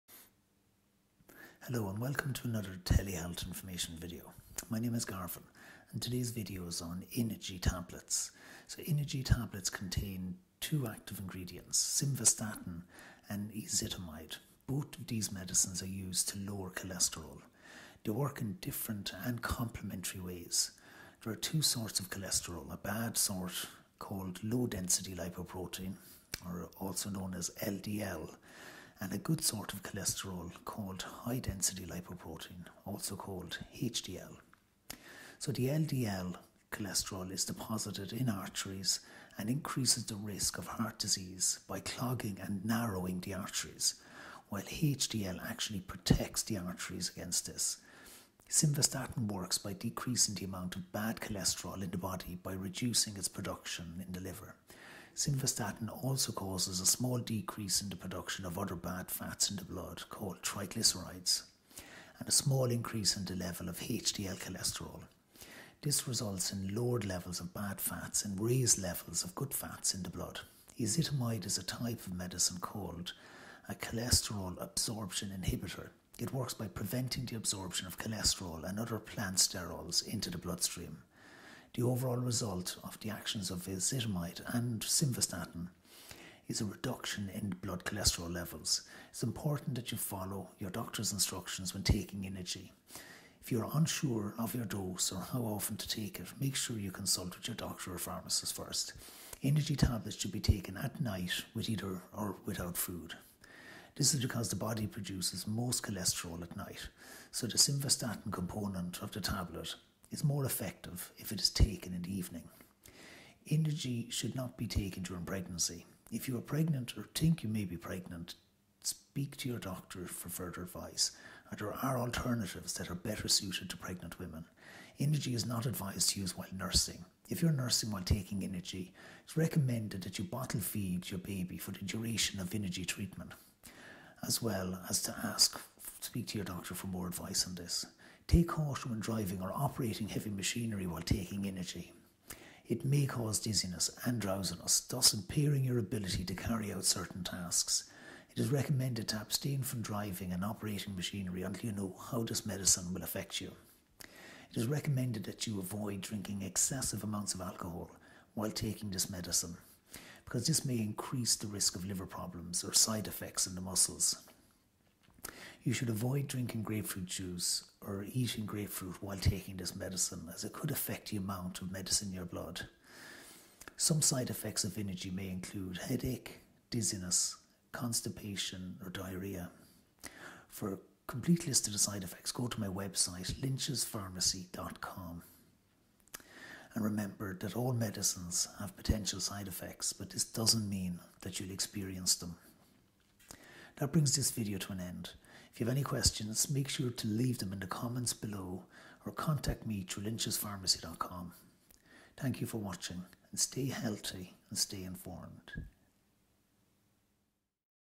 What is Inegy?
Inegy contains two active ingredients, simvastatin and ezetimibe. Both these medicines are used to lower cholesterol. They work in different and complementary ways.

There are two sorts of cholesterol; a 'bad' sort called low density lipoprotein (also known as LDL) and a 'good' sort called high density lipoprotein (also known as HDL). LDL is deposited in the arteries and increases the risk of heart disease by clogging and narrowing the arteries, while HDL actually protects the arteries against this.

Simvastatin works by decreasing the amount of ‘bad’ cholesterol in the body by reducing its production the liver.

You can also visit Lynch's Pharmacy in Broadale, Maryborough Hill, Cork.

You should always consult your doctor if you have any concerns before taking any medication.